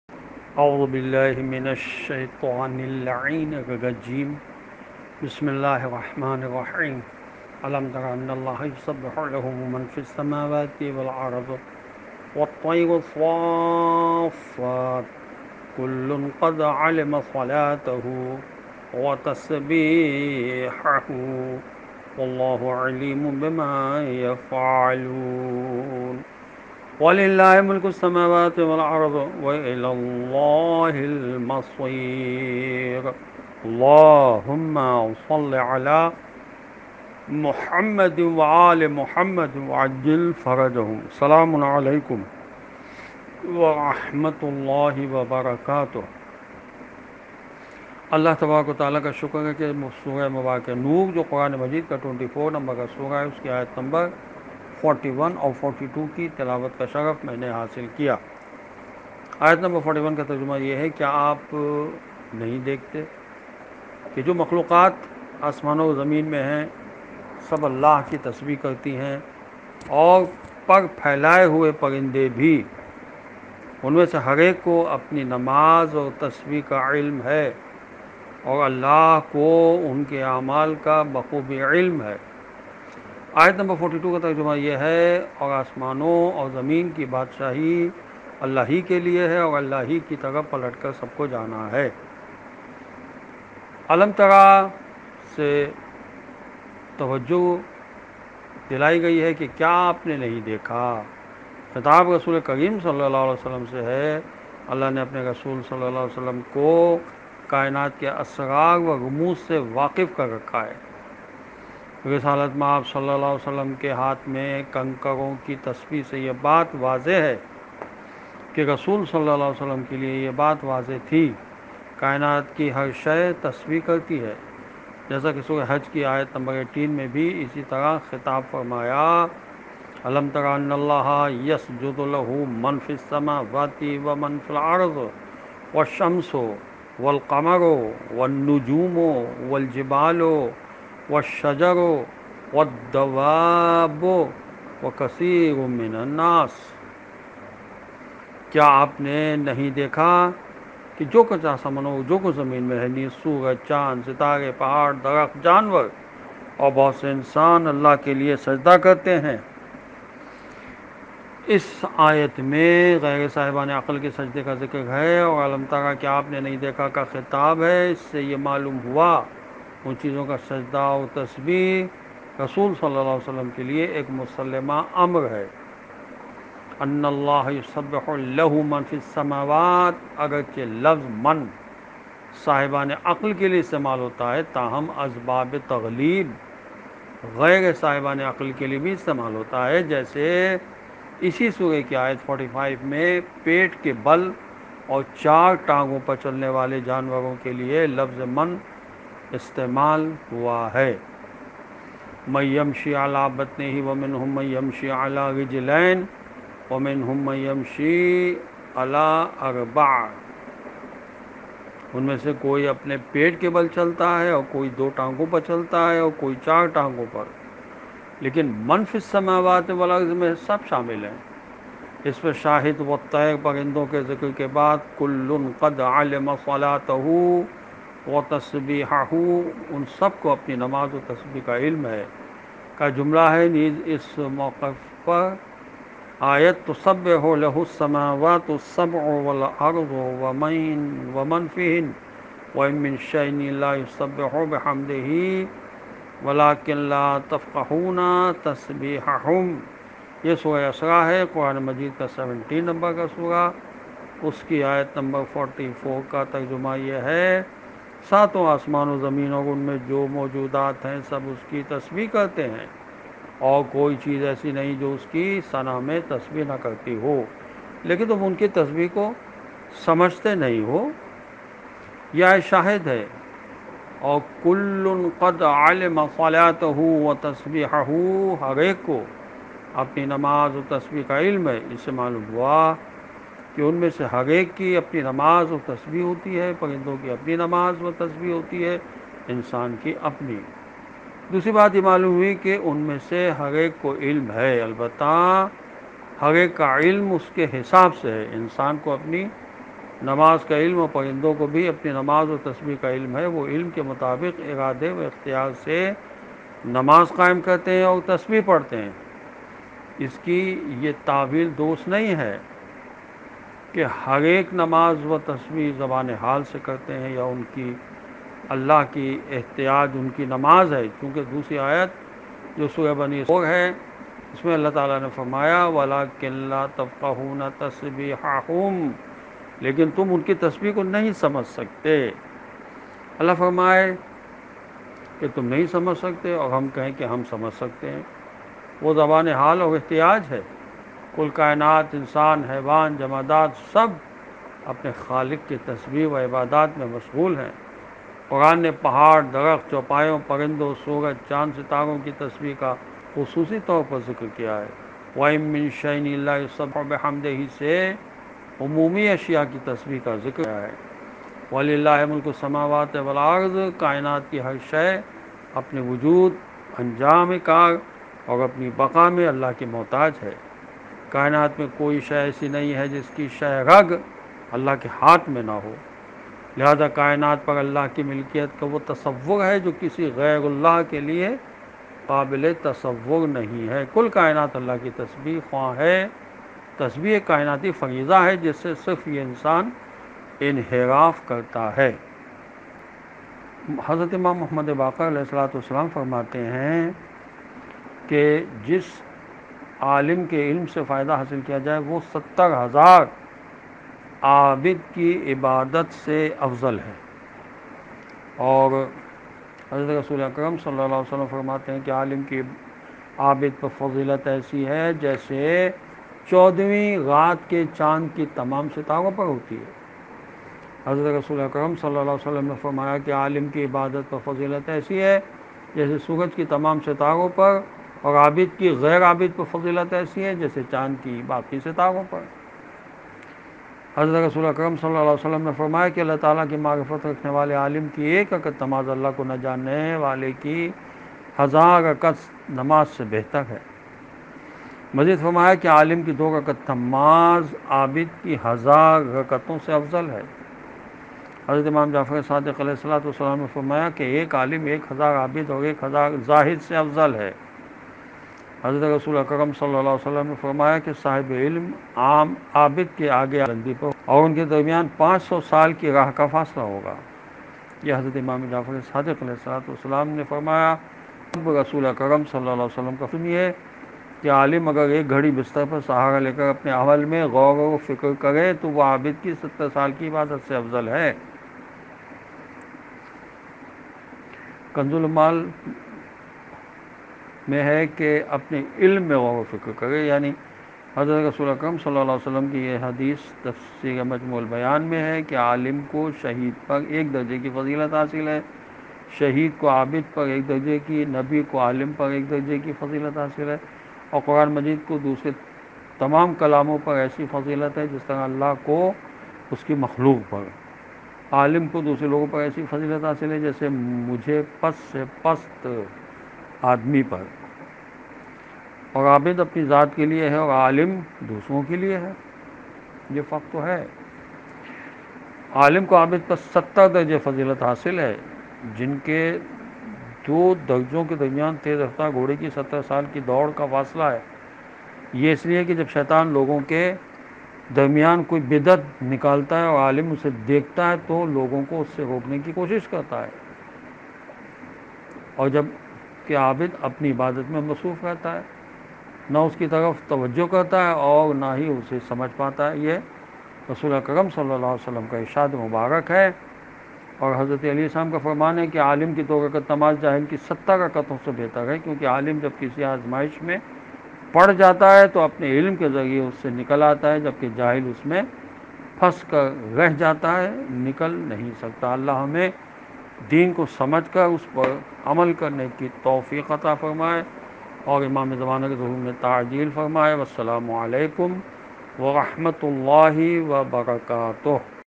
Surah Noor Verses 41-42 Urdu Translation And Tafseer By Maulana Ali Ahmed | Holy Quran Motivation
Surah Noor Verses 41-42